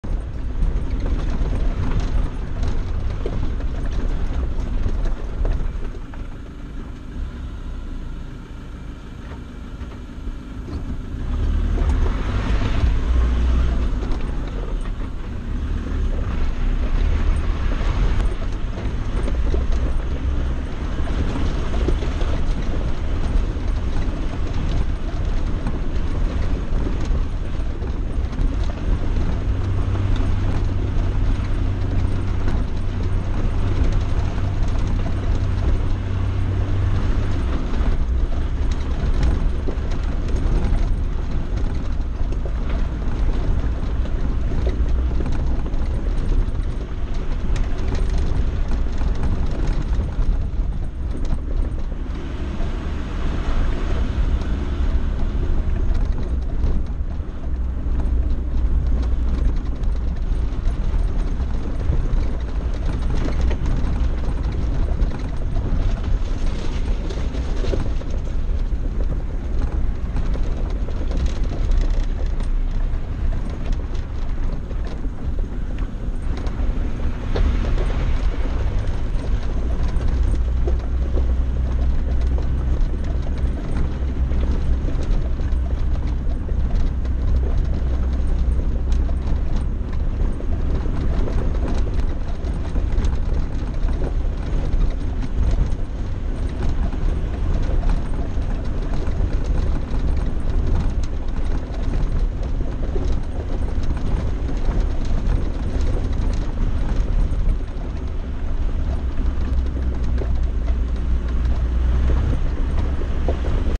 Donde es esta pista??? Alguien sabe???? Alpes? Pirineos? Francia?? Italia?? España?? Andorra???
Ya ha salido la respuesta en algún comentario, por lo que os lo pongo aquí,

Tornante della Boaria, en la strada della Via del Sale a Limone Piemonte (Cuneo)